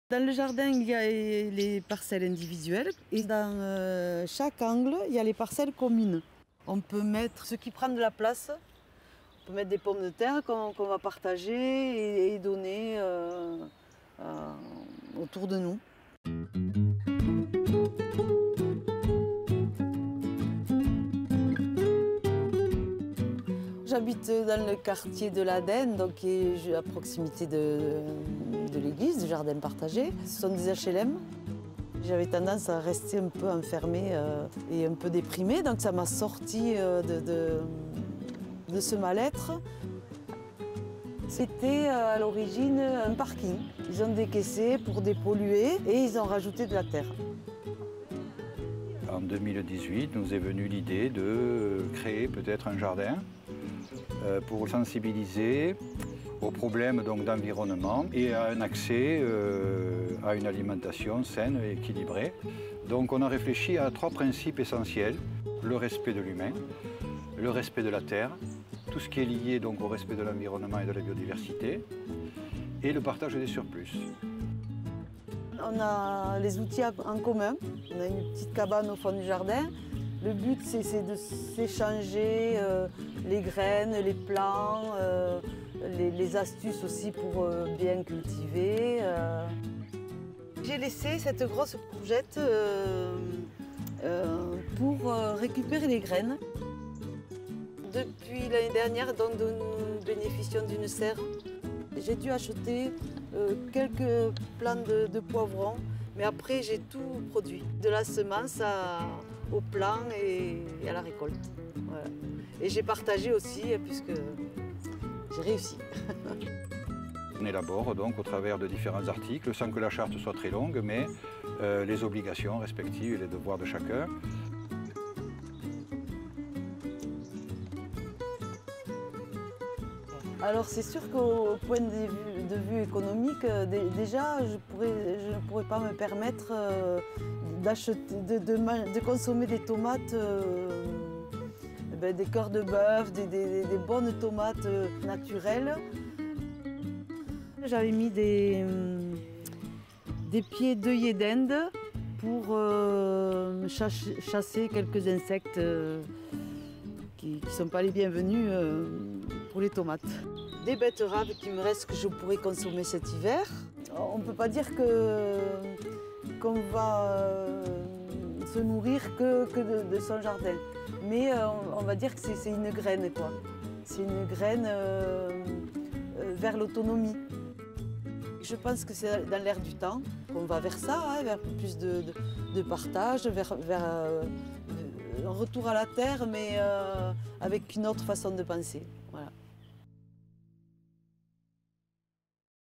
Le jardin de Castres (Tarn)
"Un retour à la terre avec une autre façon de penser", c'est ce que vit Isabelle membre active du jardin partagé de Castres